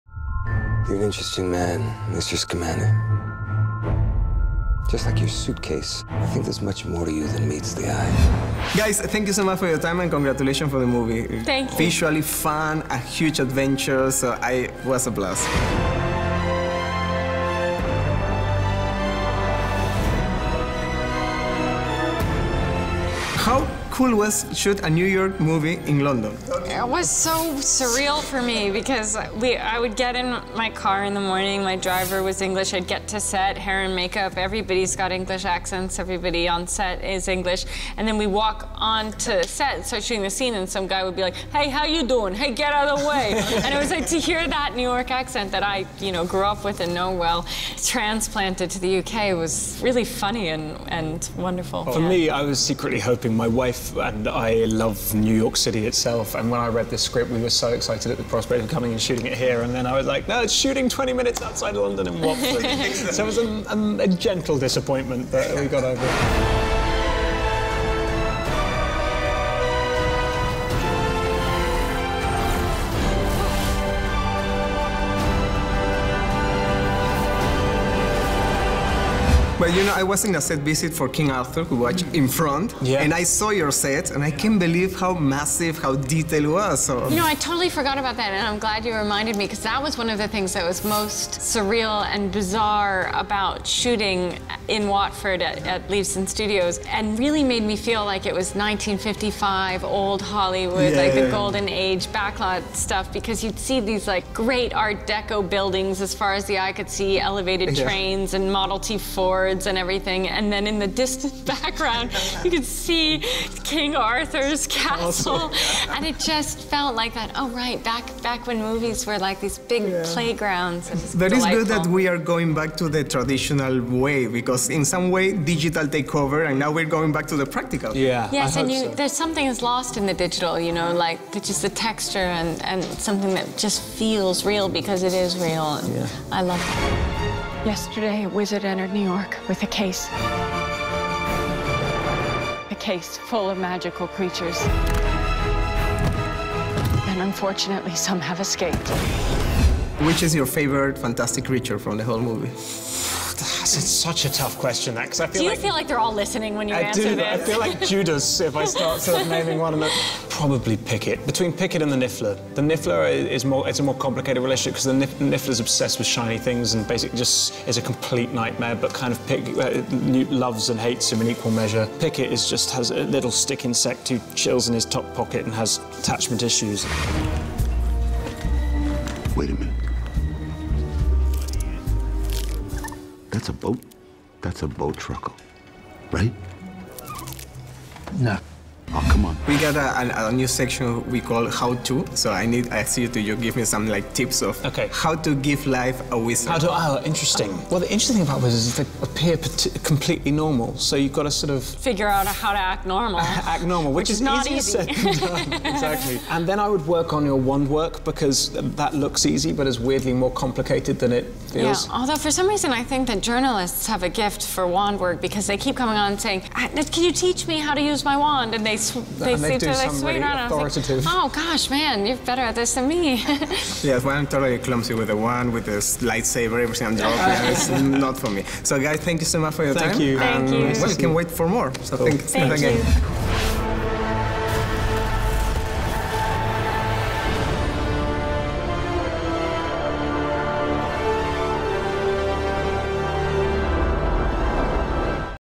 Entrevista a Eddie Redmayne y Katherine Waterstone - Animales Fantásticos
Bruno conversó con los protagonistas de 'Animales Fantásticos y Dónde Encontrarlos', Eddie Redmayne y Katherine Waterstone. No te pierdas esta divertida entrevista.